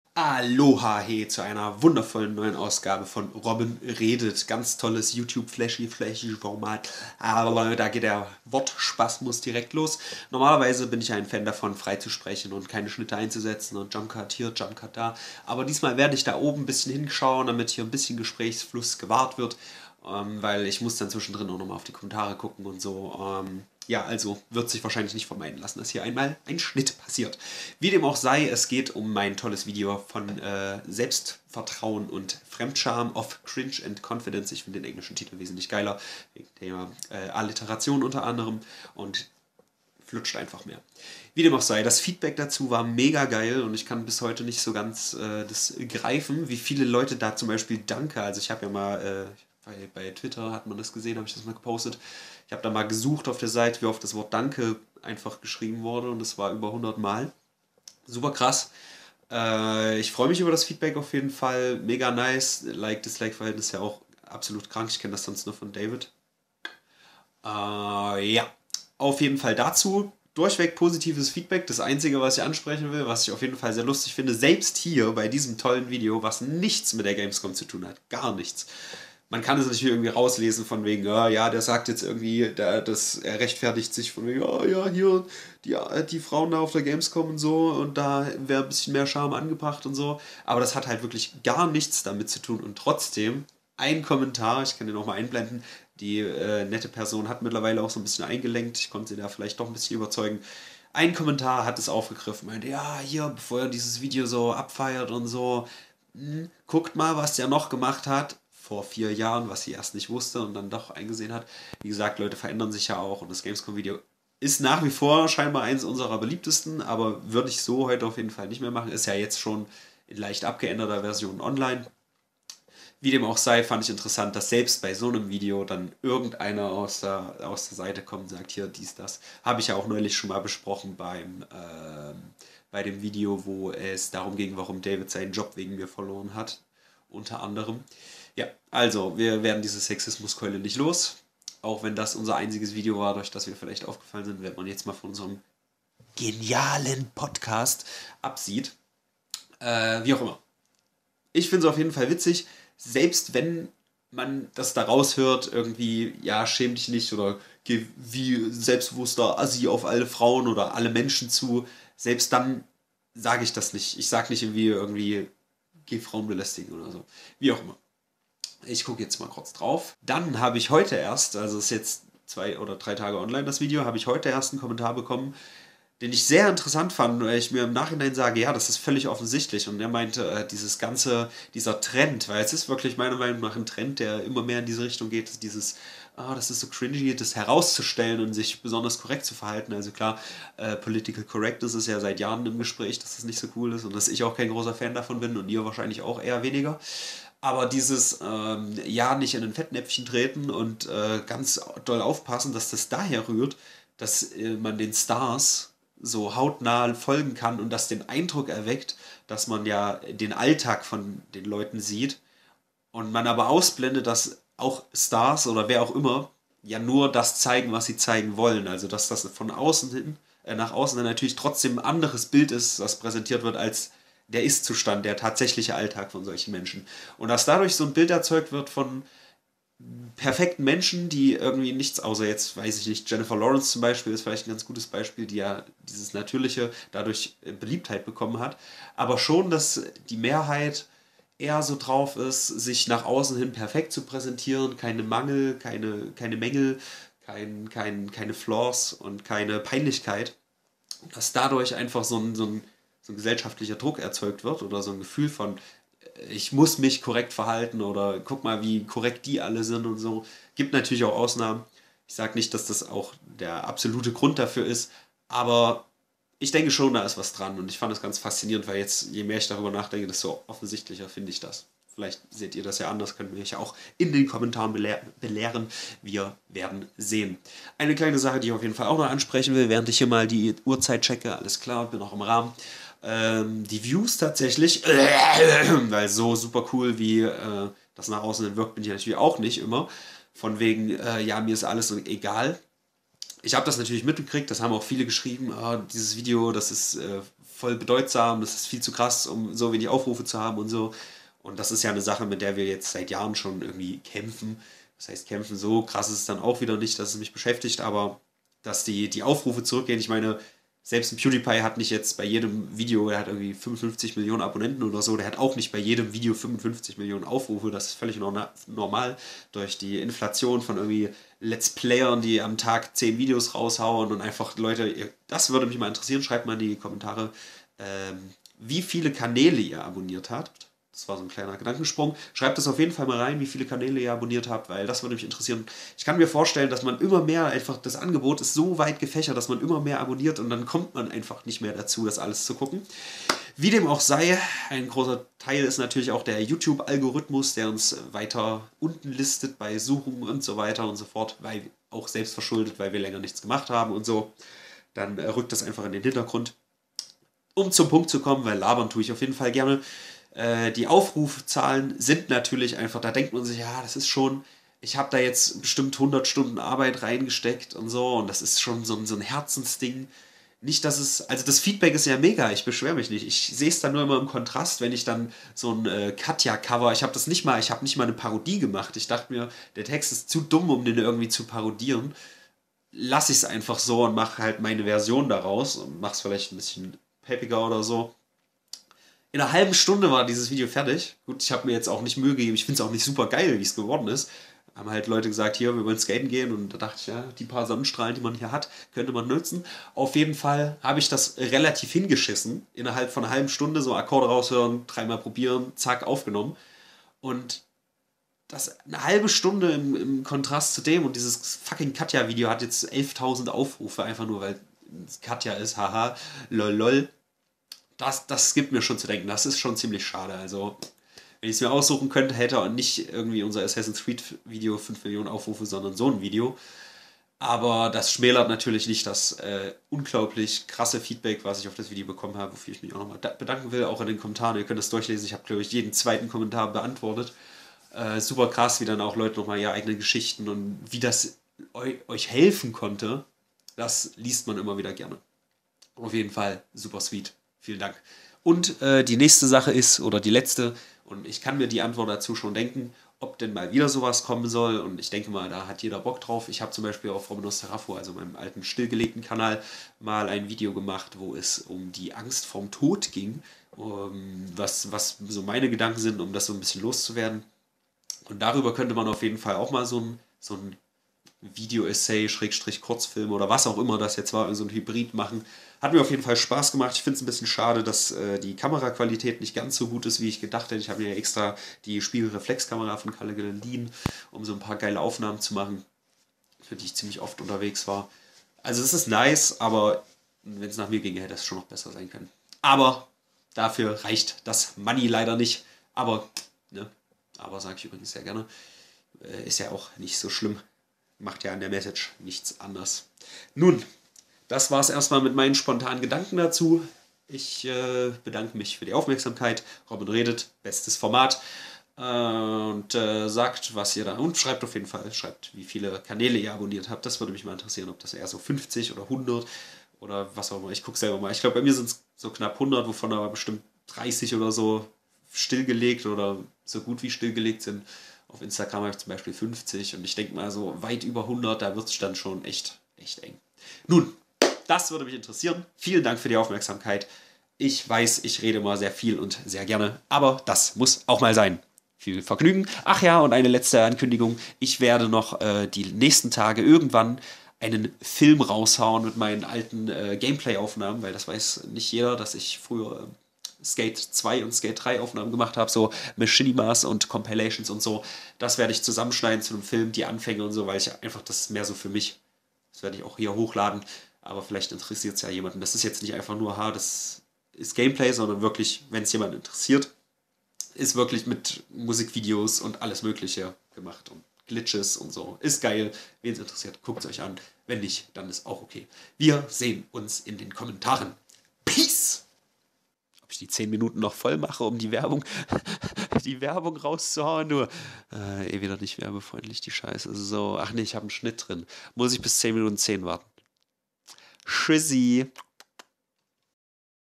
Scham, Feedback & "...of Cringe and Confidence." // Robin Redet.
Mehr über Scham, Cringe und die Sexismus-Keule, der wir seit Jahren nicht ausweichen können.